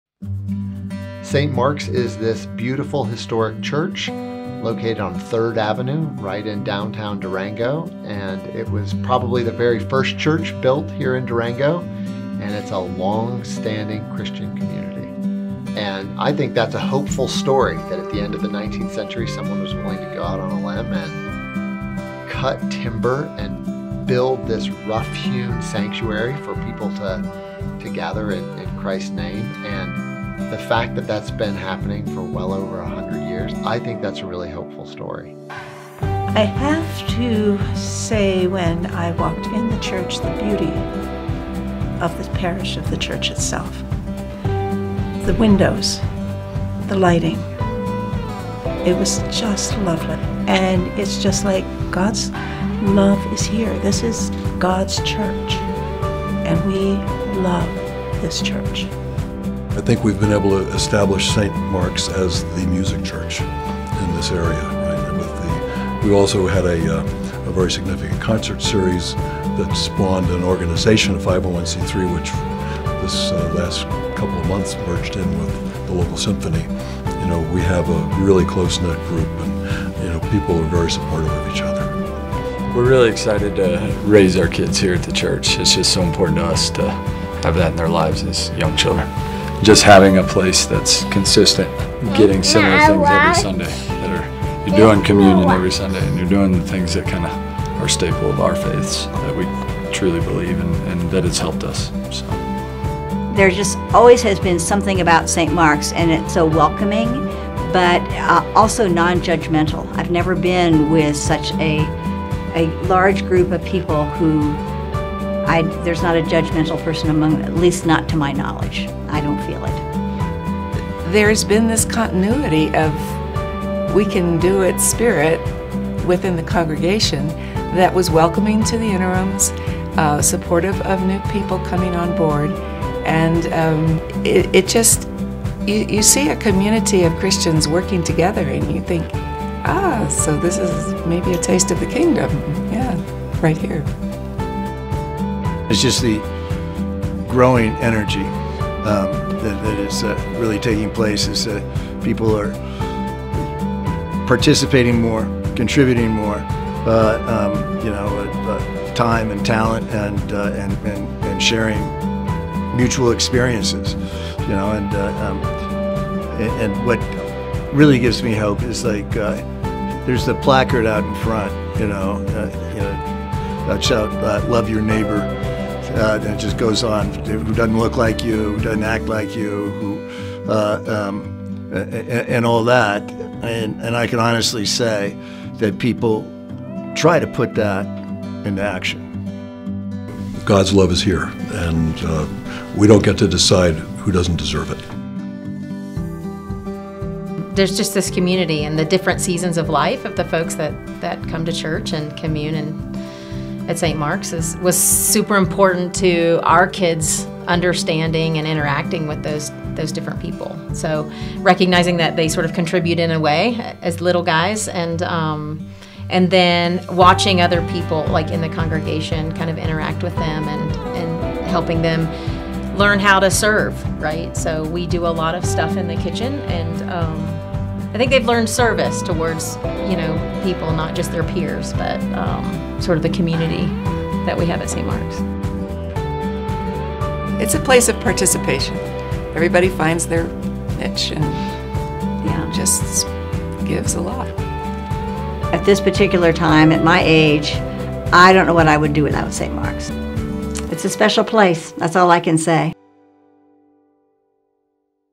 Welcome to St. Mark's Episcopal Church!
Join us for a heartfelt journey as we introduce you to our warm and welcoming faith community. Whether you're a first-time visitor or a long-standing member, this video is your invitation to experience the spirit and love that define our church family. At St. Mark's, we believe in embracing diversity, fostering spiritual growth, and extending a hand of love and friendship to all. We can't wait to meet you, and we hope this video serves as the first step on your journey to becoming a cherished member of our congregation.